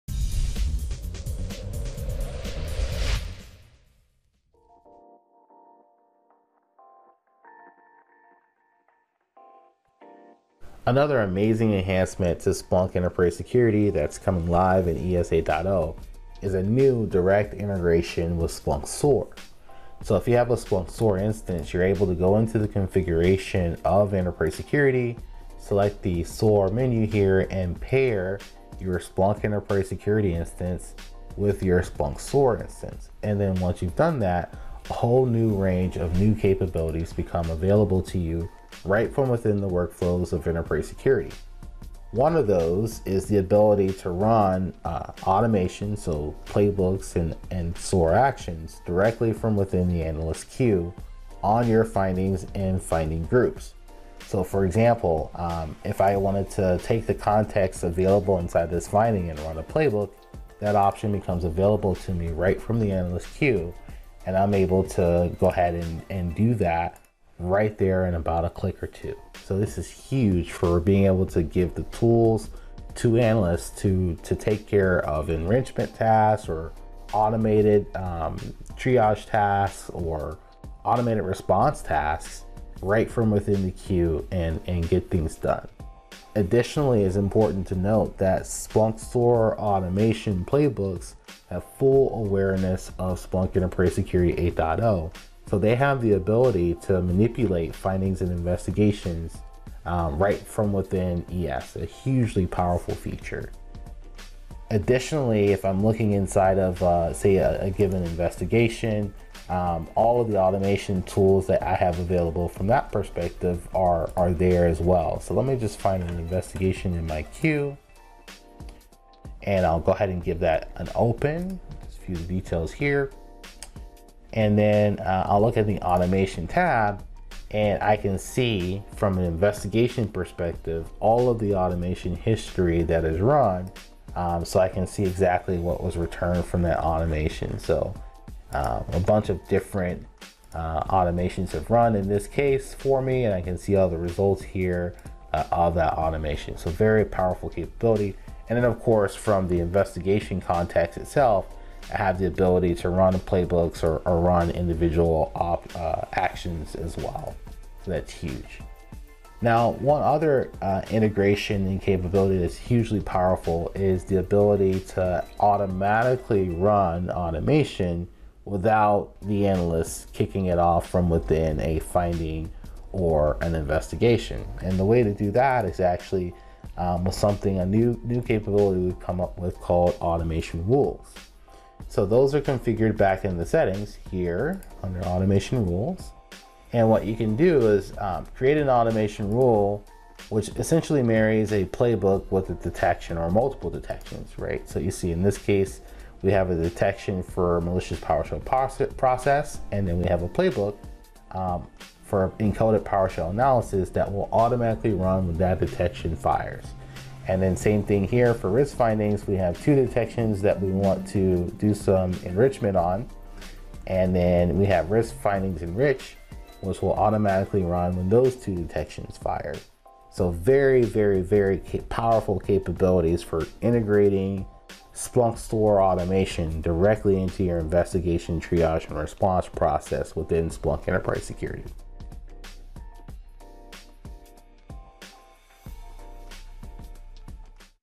Splunk Enterprise Security 8.0 - SIEM and SOAR Unified Workflows
In this SIEM in Seconds demo, see how direct integration with Splunk SOAR playbooks and actions within the case management and investigation features of Splunk Enterprise Security and Mission Control delivers a single unified work surface. Optimize mean time to detect (MTTD) and mean time to respond (MTTR) for an incident. Analysts can detect, investigate and respond to threats from one modern interface.